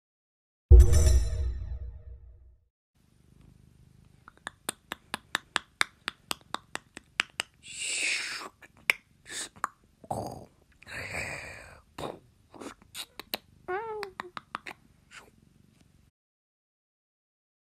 Sword Fight | Lego Stop Motion
Hey! It's me DonnyDonny. Here is my Lego Stop Motion Sword Fight video that I made.

Two twins meet at the park and want to kill each other.